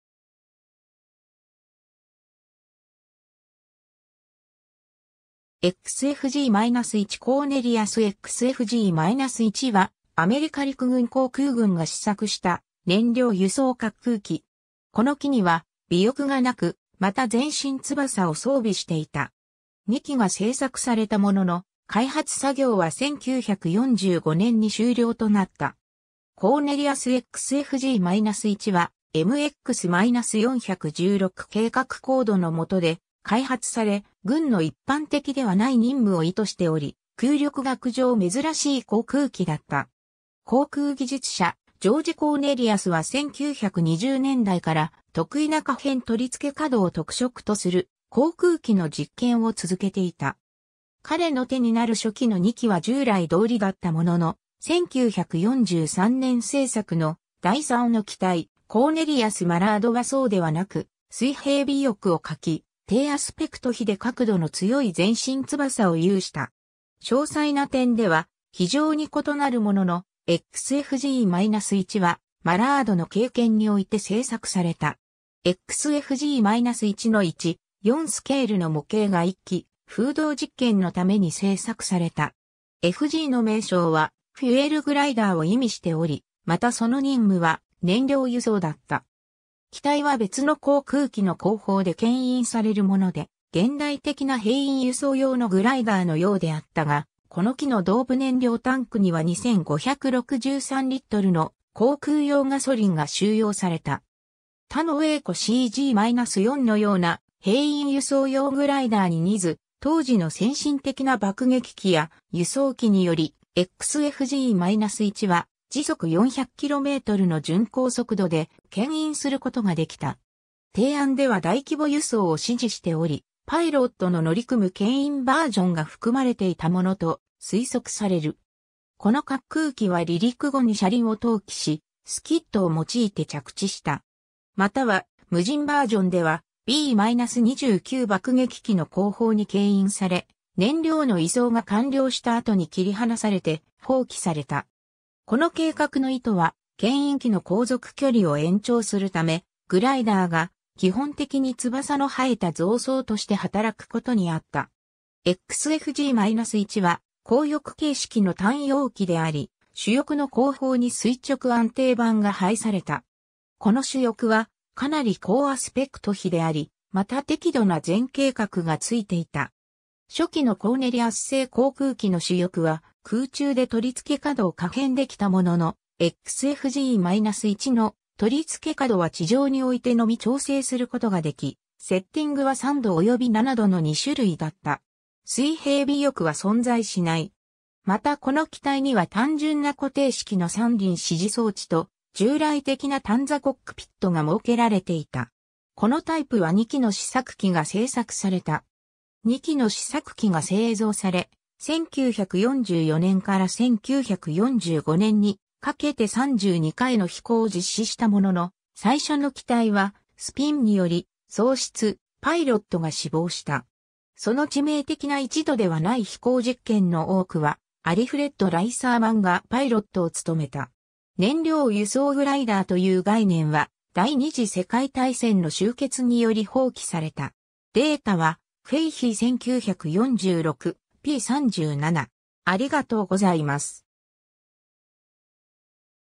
XFG-1 (航空機)
XFG-1 コーネリアス XFG-1（Cornelius XFG-1）は、アメリカ陸軍航空軍が試作した燃料輸送滑空機（軍用グライダー）。
この機には尾翼がなく、また前進翼を装備していた。
2機が製作されたものの、開発作業は1945年に終了となった。
 コーネリアス XFG-1は「MX-416」計画コードのもとで開発され、軍の一般的ではない任務を意図しており、空力学上珍しい航空機だった。
航空技術者ジョージ・コーネリアスは1920年代から、特異な可変取付け角を特色とする航空機の実験を続けていた。
彼の手になる初期の2機は従来通りだったものの、1943年製作の第3の機体コーネリアス・マラードはそうではなく、水平尾翼を欠き、低アスペクト比で角度の強い前進翼を有した。
詳細な点では非常に異なるものの、XFG-1はマラードの経験において製作された。
XFG-1の1/4スケールの模型が1機、風洞実験のために製作された。
 「FG」の名称はfuel gliderを意味しており、またその任務は燃料輸送だった。
機体は別の航空機の後方で牽引されるもので、現代的な兵員輸送用のグライダーのようであったが、この機の胴部燃料タンクには2,563リットルの航空用ガソリンが収容された。
他のWaco CG-4のような兵員輸送用グライダーに似ず、当時の先進的な爆撃機や輸送機により、XFG-1は400km/hの巡航速度で牽引することができた。
 提案では大規模輸送を支持しており、パイロットの乗り組む牽引バージョンが含まれていたものと推測される。
この滑空機は離陸後に車輪を投棄し、スキッドを用いて着地した。
または無人バージョンではB-29爆撃機の後方に牽引され、燃料の移送が完了した後に切り離されて放棄された。
この計画の意図は、牽引機の航続距離を延長するため、グライダーが基本的に翼の生えた増槽として働くことにあった。
 XFG-1は高翼形式の単葉機であり、主翼の後方に垂直安定板が配された。
この主翼はかなり高アスペクト比であり、また適度な前傾角がついていた。
初期のコーネリアス製航空機の主翼は空中で取付け角を可変できたものの、XFG-1の取付け角は地上においてのみ調整することができ、セッティングは3度及び7度の2種類だった。
水平尾翼は存在しない。
またこの機体には単純な固定式の三輪支持装置と、従来的な単座コックピットが設けられていた。
このタイプは2機の試作機が製作された。
 2機の試作機が製造され、1944年から1945年にかけて32回の飛行を実施したものの、最初の機体はスピンにより喪失、パイロットが死亡した。
その致命的な1度ではない飛行実験の多くはアリフレッド・ライサーマンがパイロットを務めた。
燃料輸送グライダーという概念は第二次世界大戦の終結により放棄された。
 データはFahey 1946, p. 37。